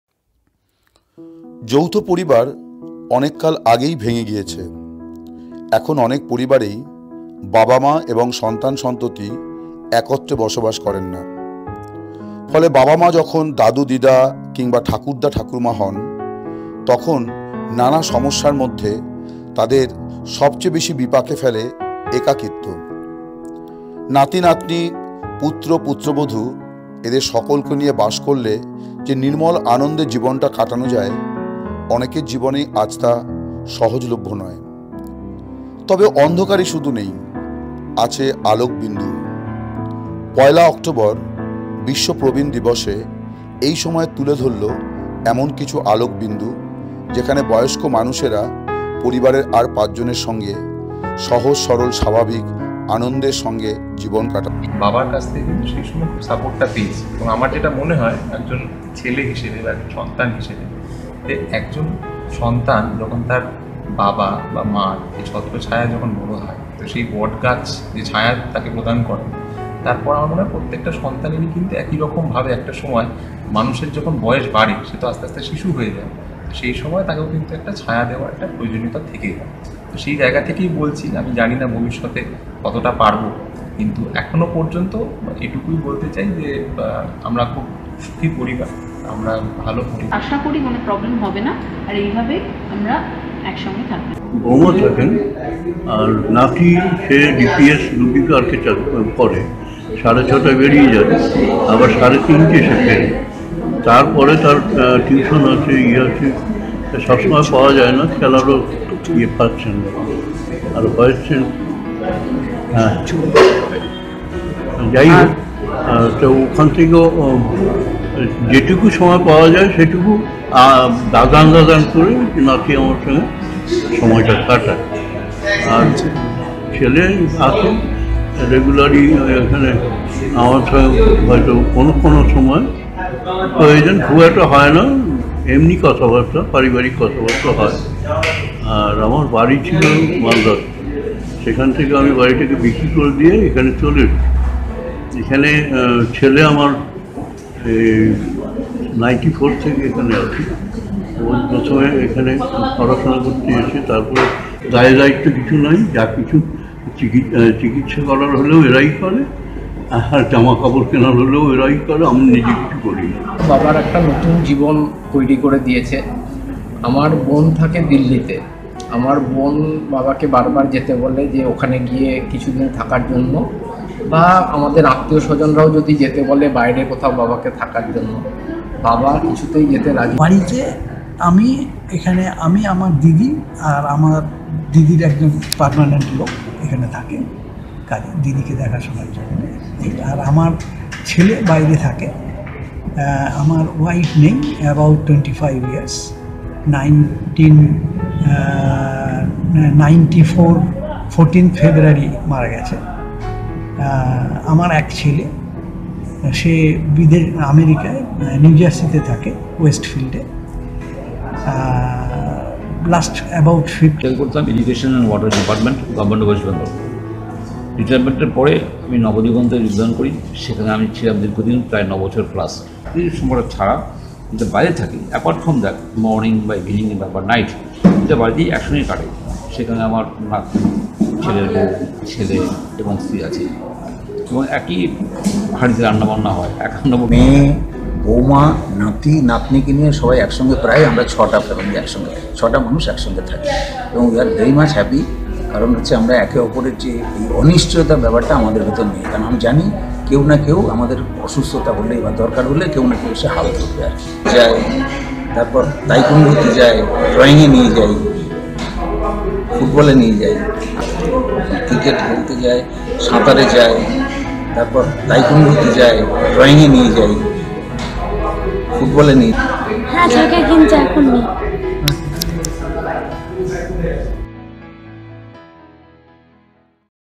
একাকী নয়, যৌথ পরিবারেই বেঁচে এই প্রবীণরা | International Day for Elderly People
যৌথ পরিবার ভেঙে গিয়েছে বহুকাল। পুত্র-পূত্রবধূ, নাতি-নাতনিদের নিয়ে একত্রে বসবাসও কমে আসছে। বিশেষ করে শহর এলাকায়। আবার একত্রে থাকার সুবাদেই মানসিক ও শারীরিক নির্যাতনের শিকার বহু প্রবীণ। একাকিত্ব থেকে বাঁচতে আত্মহত্যার পথ বেছে নিচ্ছেন অনেকেই। কিন্তু সবটাই অন্ধকার নয়। ১ অক্টোবর, বিশ্ব নাগরিক দিবসে নিভে না যাওয়া সেই সব আলোকবিন্দুগুলিকে তুলে ধরল এই সময়